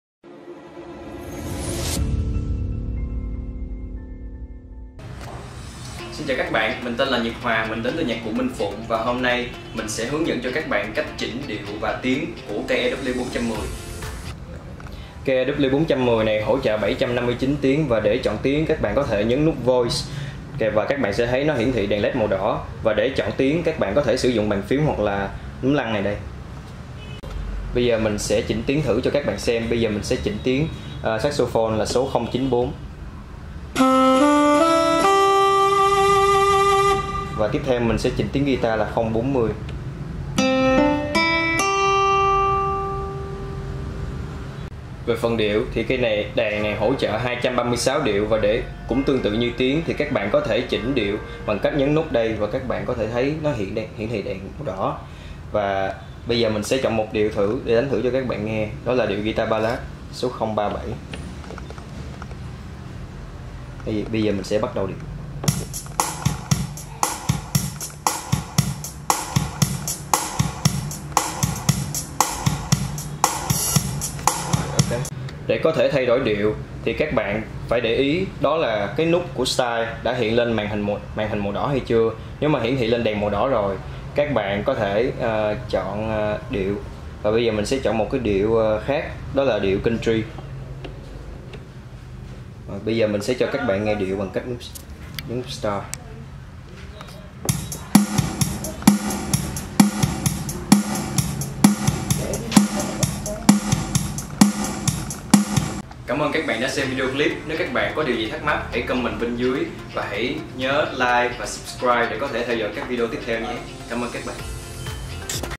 Bài 1:Thao tác chọn điệu tiếng ||Hướng dẫn Organ EW410 || Nhạc Cụ Minh Phụng
Đàn Organ Yamaha EW410 là một Model mới nhất của Yamaha với dòng đàn học sinh được ưa chuộng nhất năm 2018.